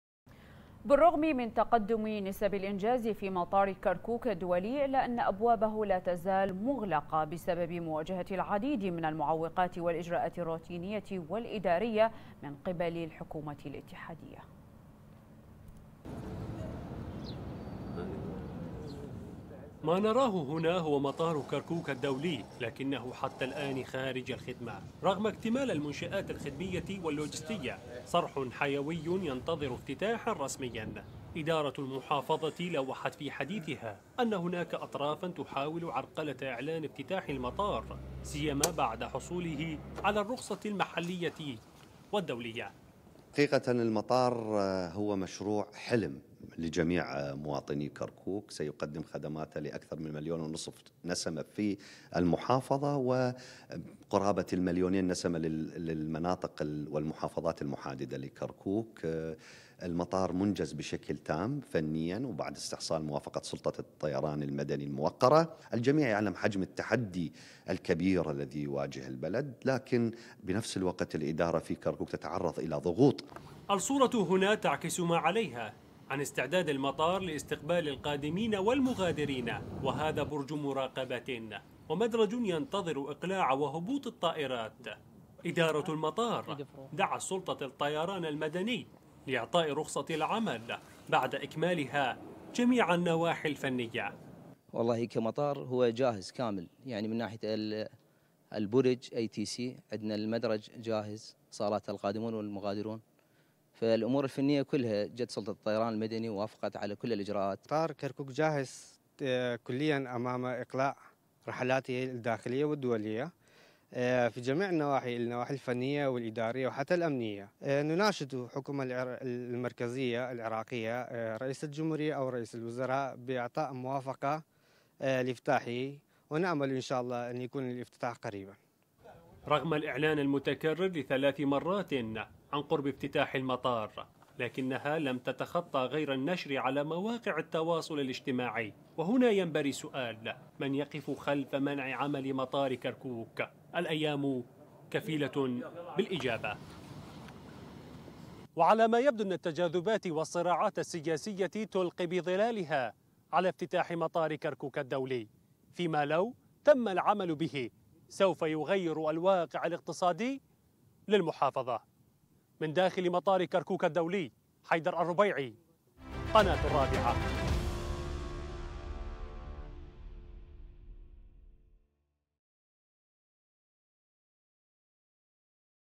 الإجراءات الحكومية تؤخر افتتاح بوابات مطار محافظة كركوك الدولي
الإجراءات الحكومية تؤخر افتتاح بوابات مطار محافظة كركوك الدولي.. ونسب الإنجاز تعلن انتهاء جميع متطلباته الفنية واللوجستية